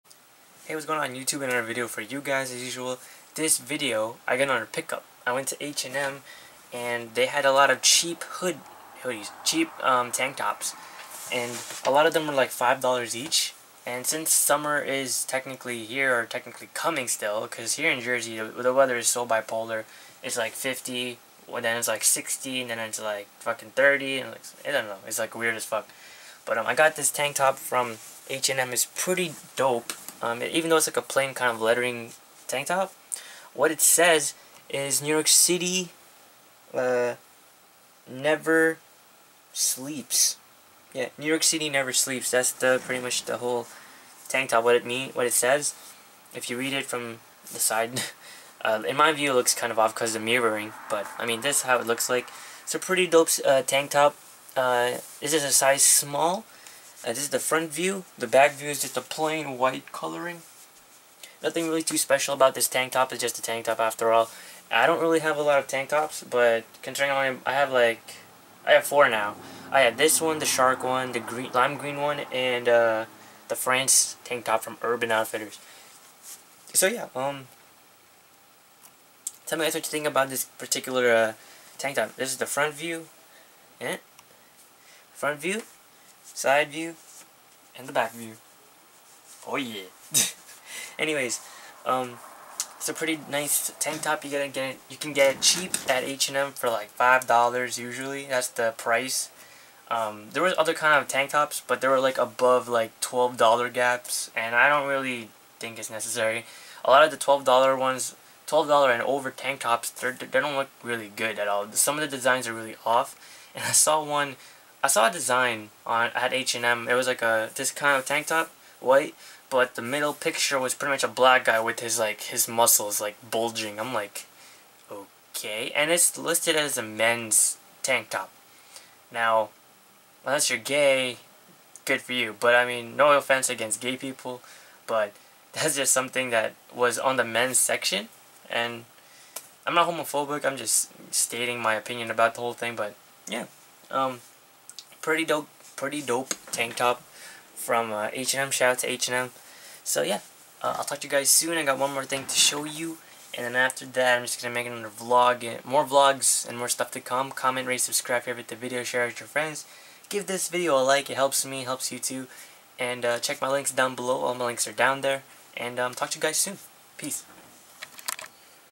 Bought A Printed White New York City Never Sleeps Tank Top At H&M :)
$5